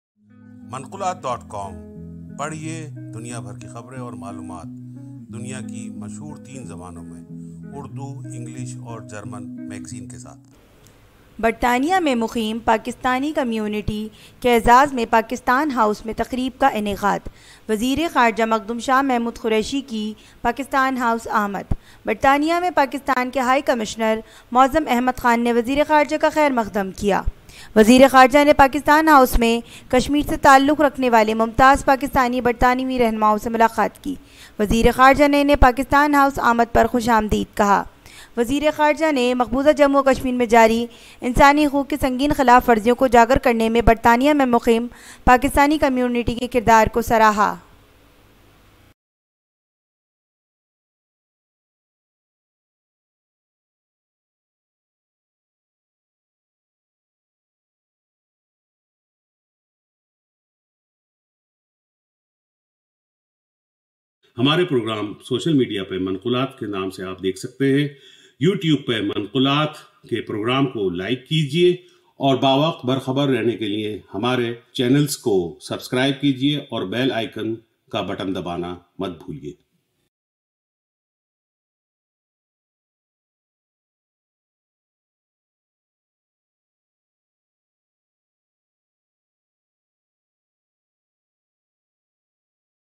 Foreign Minister Makhdoom Shah Mehmood Qureshi arrives at "Pakistan House"
A ceremony was held at "Pakistan House" in honor of the Pakistani community in the UK